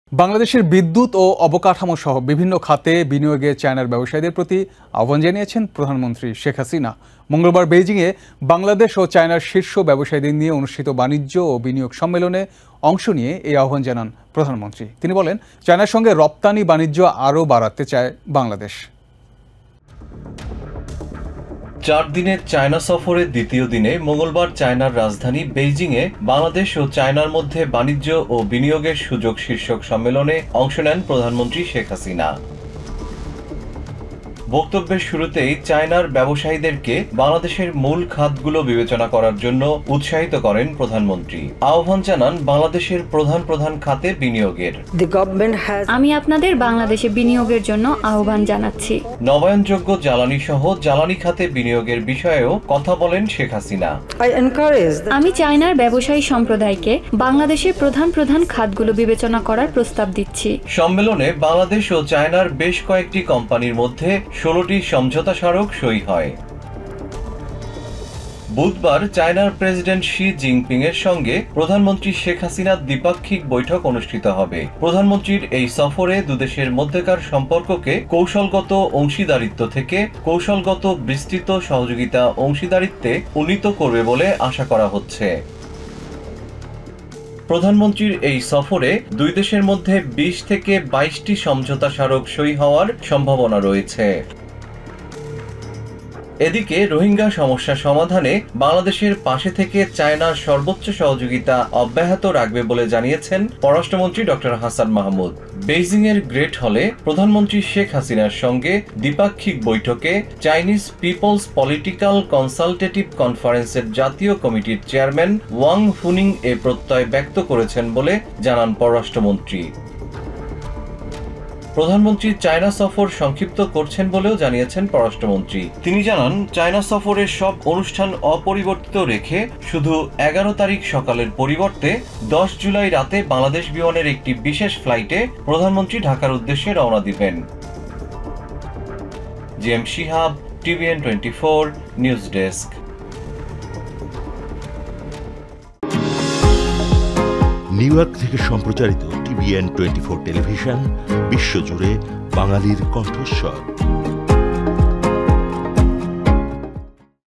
চায়নাকে বাংলাদেশের প্রধান খাতগুলোতে বিনিয়োগের প্রস্তাব | TBN24 NEWS| China | Bangladesh | Investment
বাংলাদেশের বিদ্যুৎ ও অবকাঠামোসহ বিভিন্ন খাতে বিনিয়োগে চায়নার ব্যবসায়ীদের প্রতি আহ্বান জানিয়েছেন প্রধানমন্ত্রী শেখ হাসিনা। মঙ্গলবার বেইজিংয়ে বাংলাদেশ ও চায়নার শীর্ষ ব্যবসায়ীদের নিয়ে অনুষ্ঠিত বাণিজ্য ও বিনিয়োগ সম্মেলনে অংশ নিয়ে এই আহ্বান জানান প্রধানমন্ত্রী।